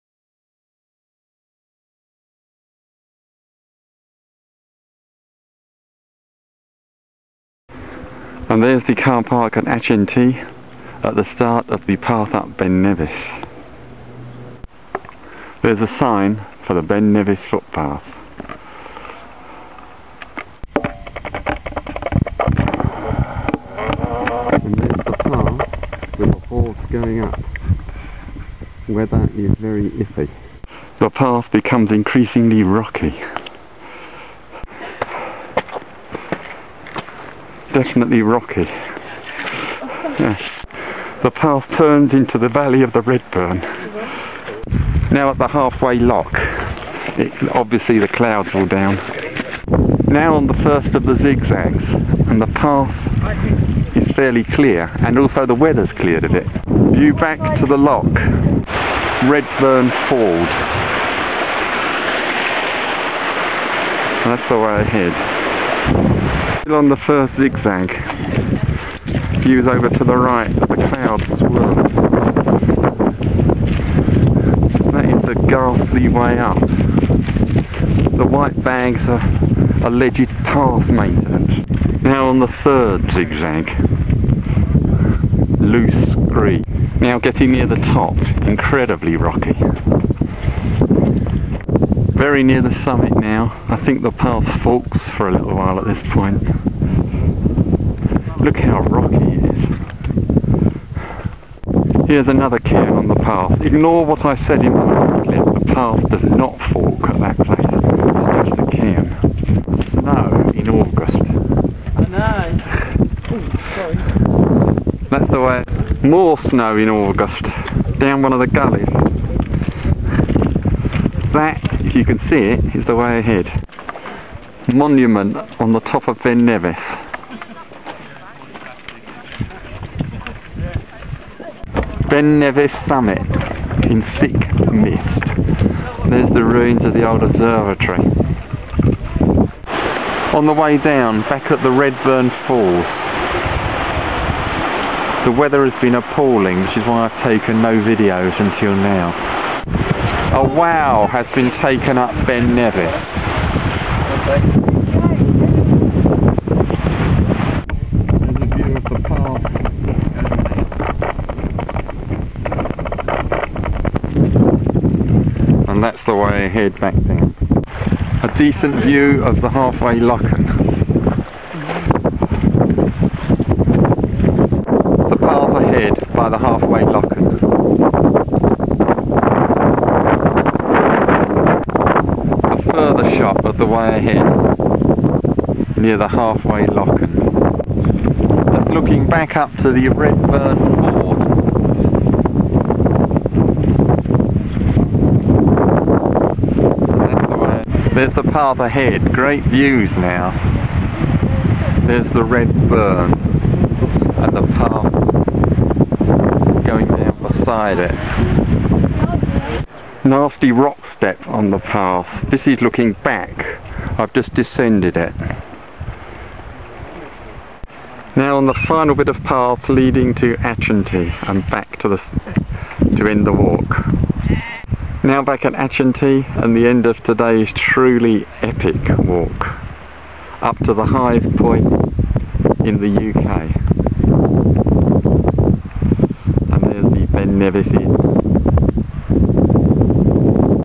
Munro bagging- Ben Nevis by the Mountain Track 8 August 2009
The ascent of Ben Nevis, the highest point in Britain,  by the Mountain Track from Achintee. This "there and back" walk is 9 miles long (around 13 miles to and from Fort William). The weather was very bad at times during the walk which limited the amount of videoing possible. A big thank you to the friendly couple from the Isle of Wight and their two children who kept me company and helped me over some difficult bits on the way down.